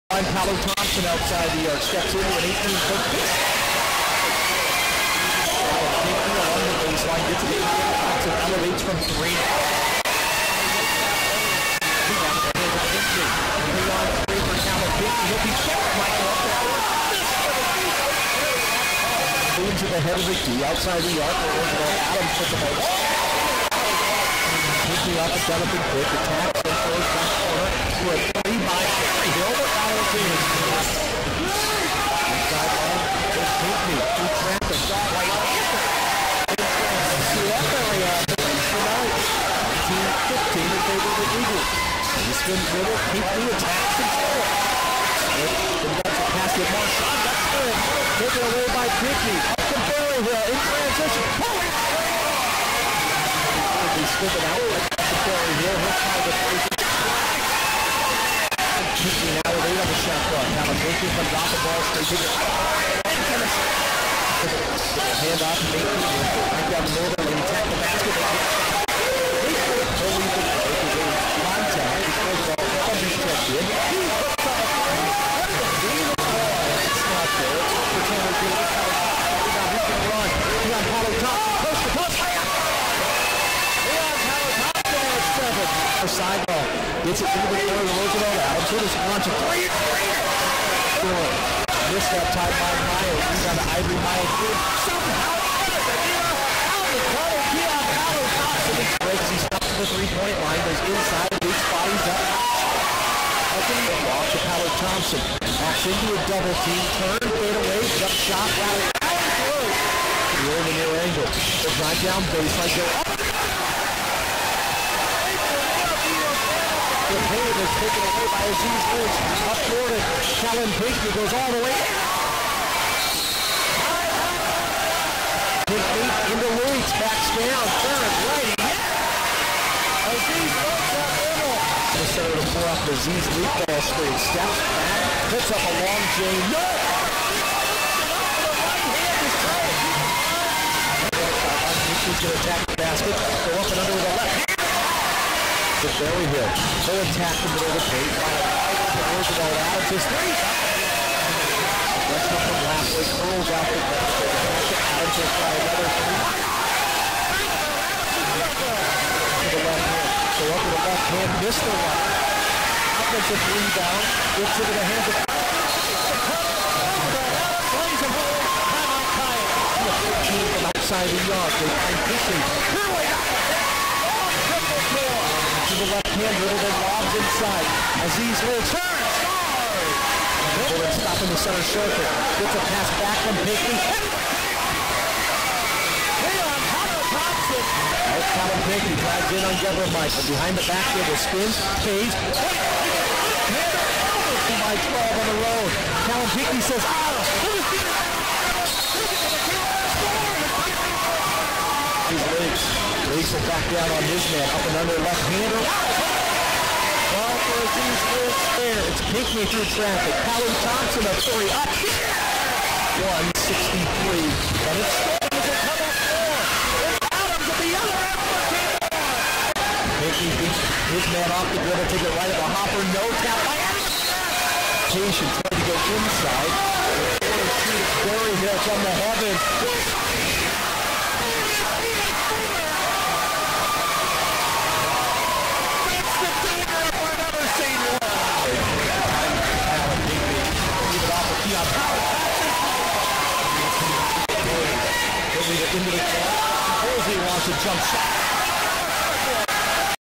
Men's Basketball - College of Idaho 95, Northwest 73
Highlights of College of Idaho's 95-73 win at Northwest University (audio courtesy of ESPN Boise)